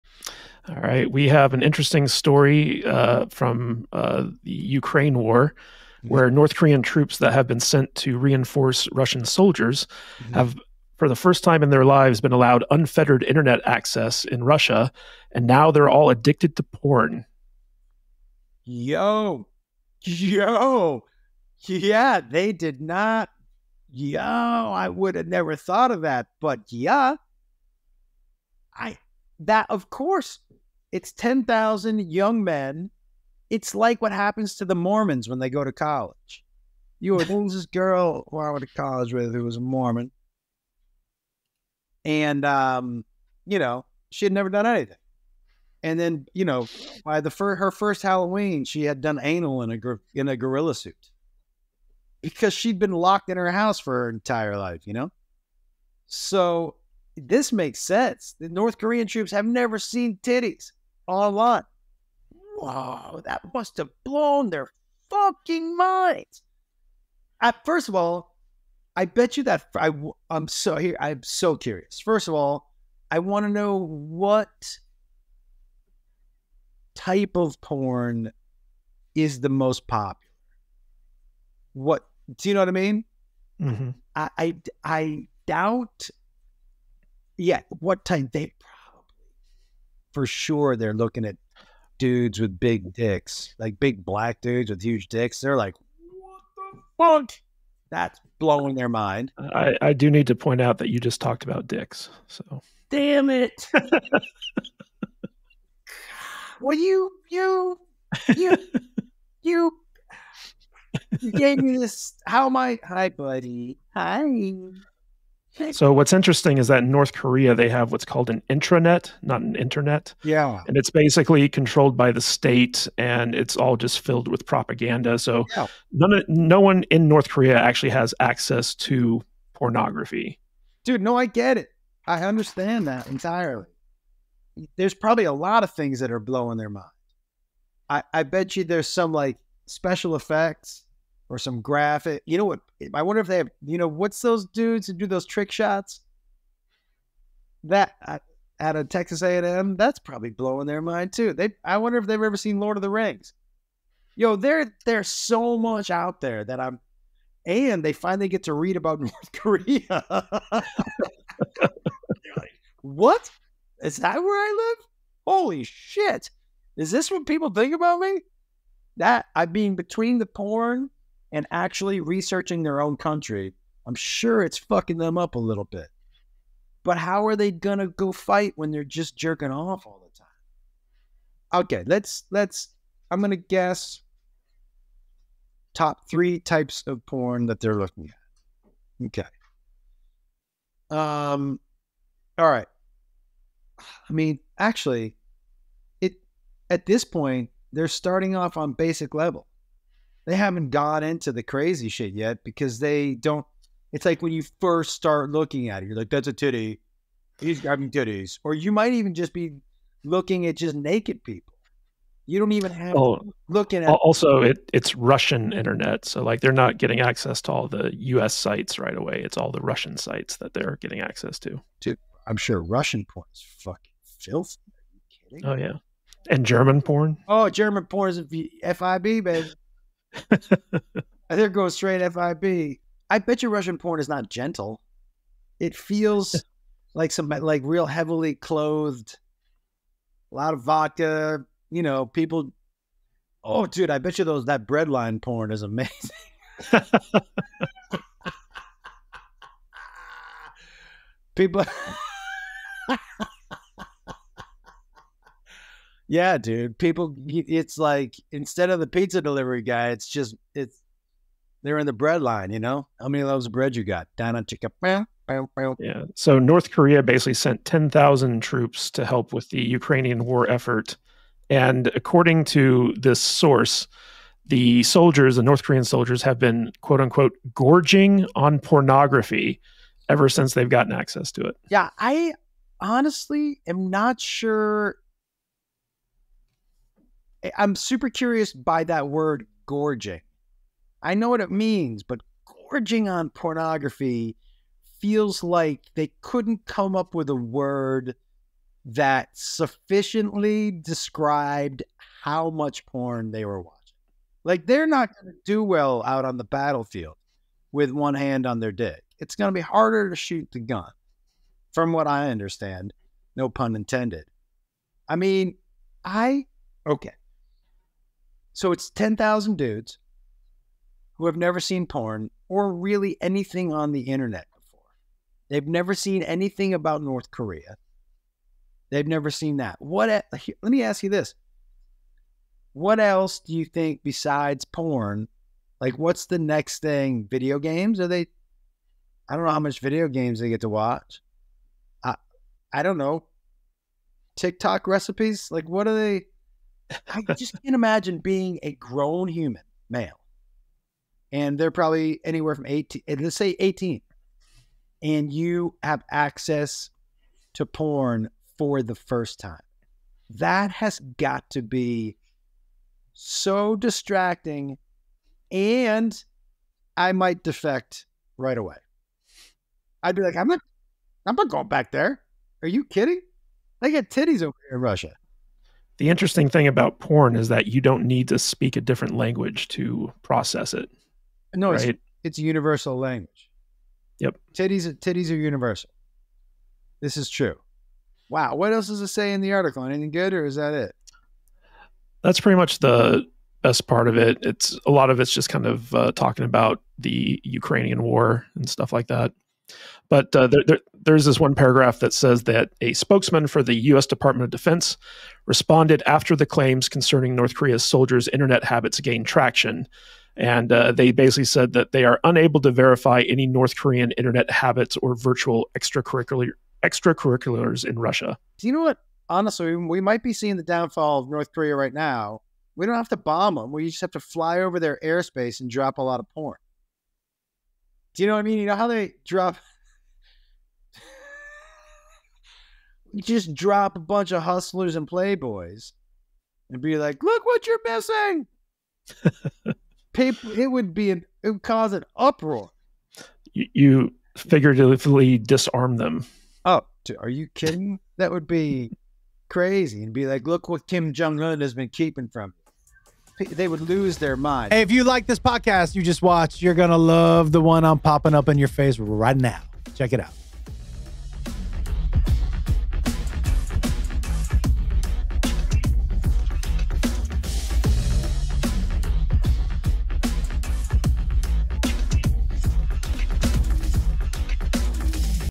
The North Korean Army Gets Addicted To Pr0n!
You can't make this up - the North Korean army is dispatched to help Russia fight the war in the Ukraine, and as soon as they get unrestricted internet access, all the soldiers want to do is look at adult websites!  Well, that's one way for peace to break out.